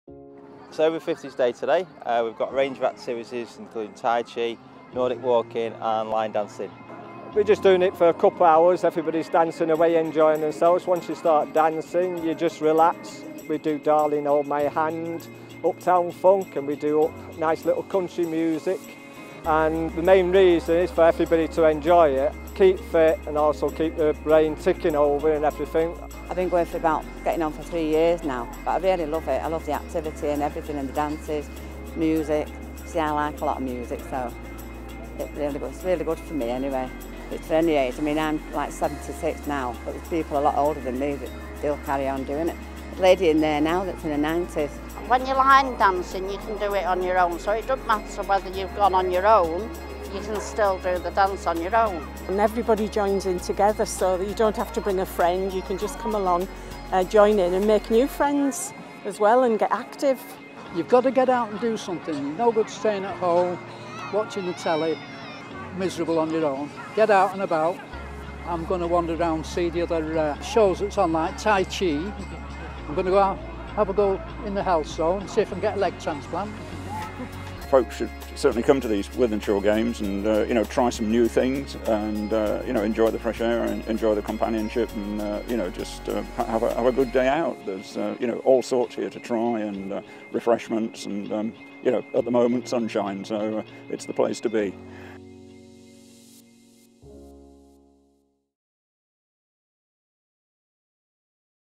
Wythenshawe Games Over 50s Day
A range of sports and activities for over 50s, including dance, nordic walking, yoga, bowls, tennis, croquet, martial arts, badminton and walking football. Join us for the 2017 Games 26th-31st July!